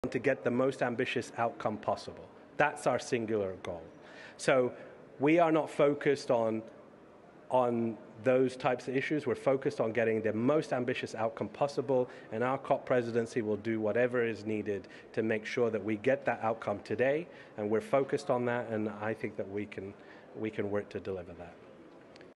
We all want to get the most ambitious outcome possible.
We all want to get the most ambitious outcome possible. Our COP Presidency will do whatever is needed to get that today.